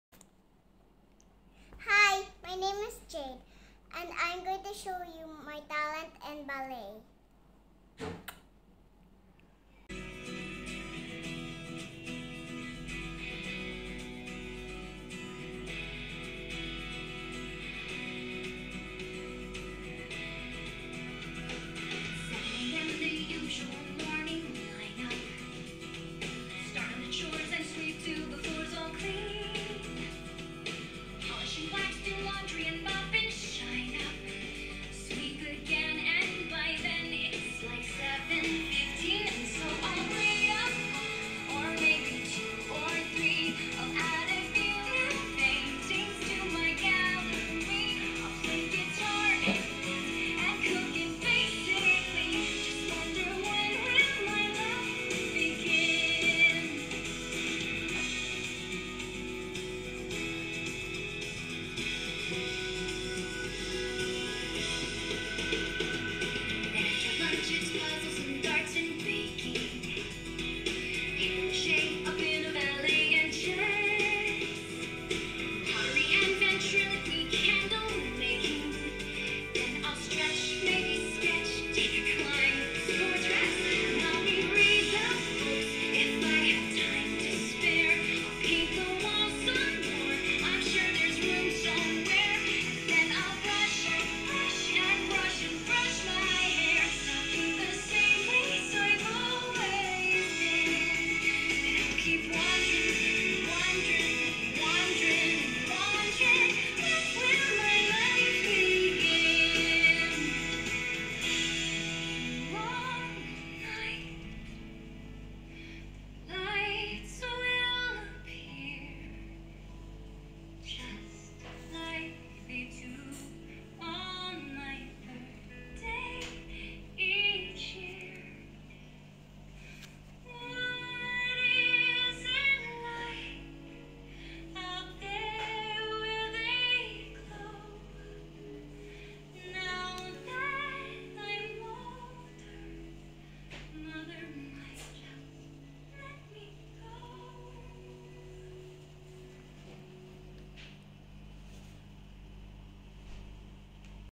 My Talent
Jade showcase her talent in dancing ballet.